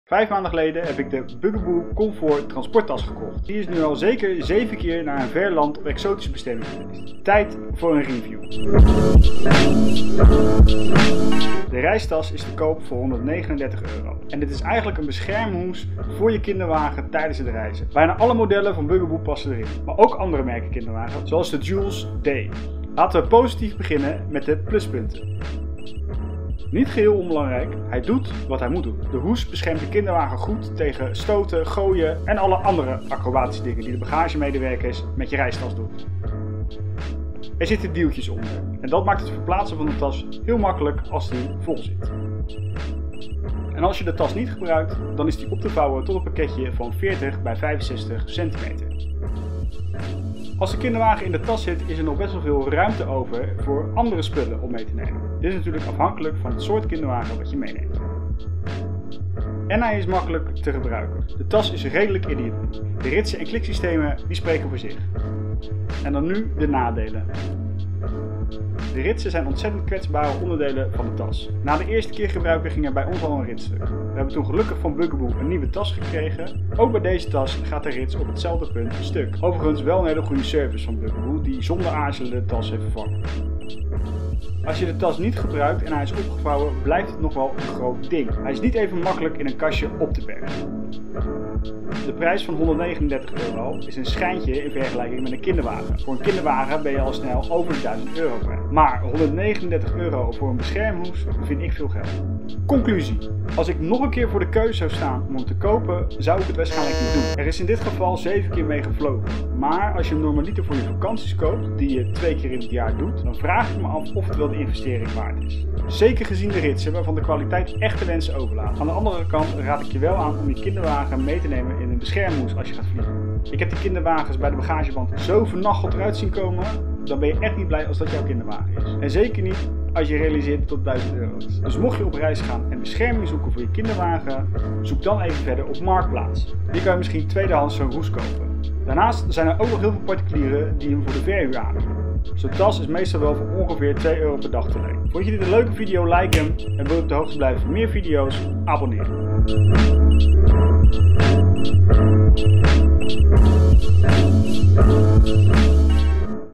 Review Bugaboo Reistas Papa's perceptie Afl.13
In deze aflevering review ik de Bugaboo Comfort reistas. Is hij echt zo handig om mee te nemen en is hij zijn geld waard ten opzichte van de kwaliteit?

Wil je de bugaboo reistas huren? link hieronder

Als aankomende vader heb je geen idee wat je te wachten staat. Je hoort de verhalen maar je weet gewoon niet wat je kunt verwachten.

In deze video's deel ik mijn ervaring als beginnend vader.